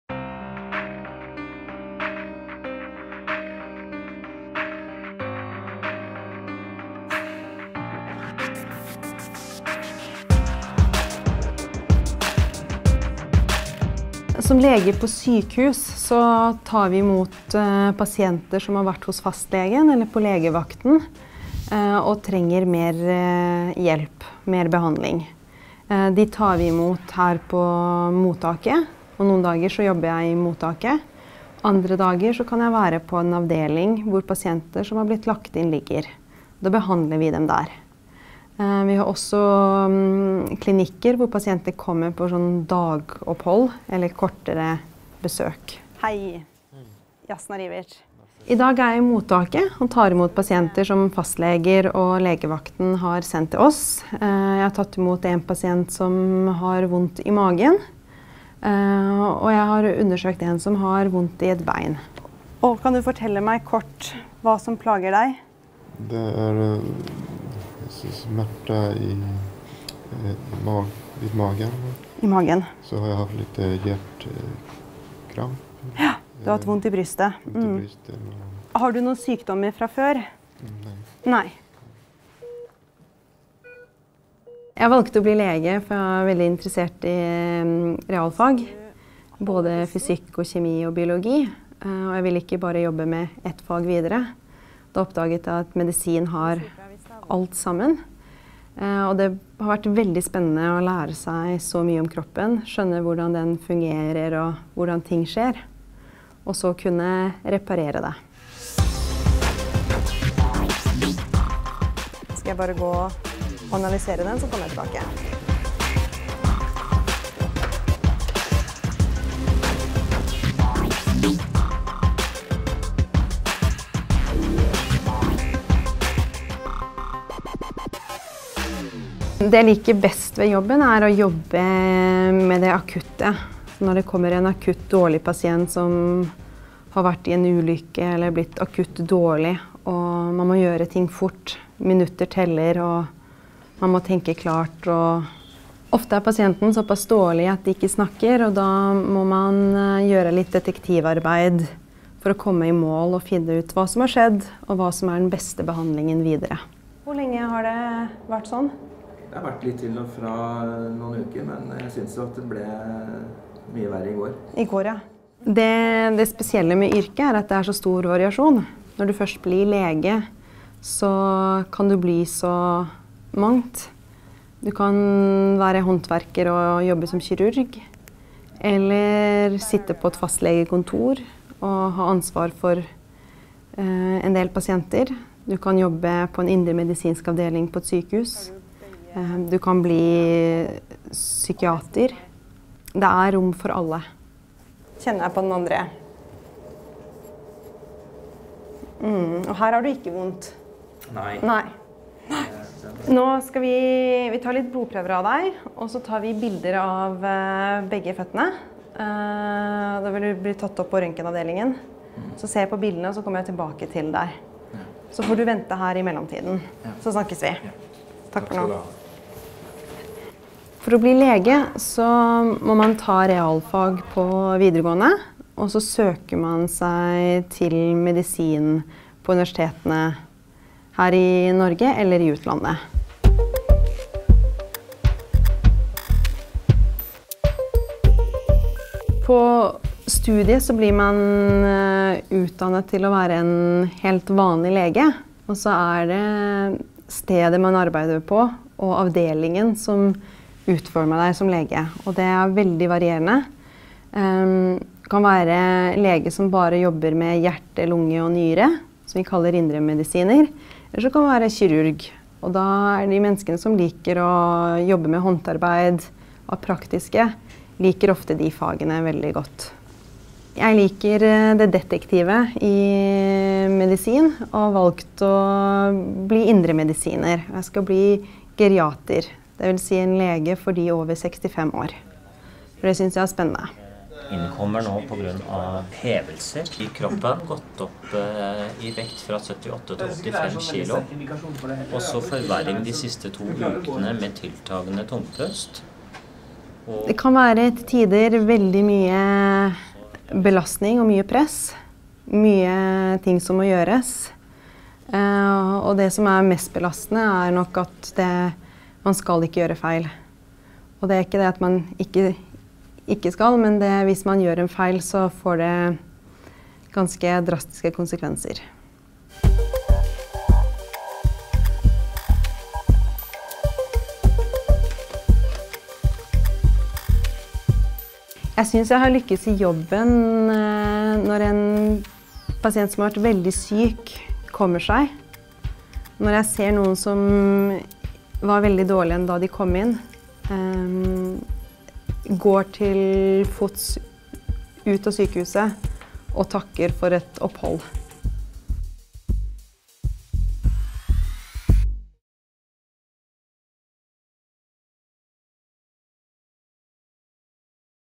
Legeyrket
Her kan du bli litt bedre kjent med vår unge, lovende LIS-lege Jasna Ribic, som forteller om legeyrket. Målgruppen for denne filmen er ungdom som skal velge yrkeskarriére.

Filmet av Andrea Gálfi og Frode Stangeland fra Pareto Film AS / Fagfilm (2015).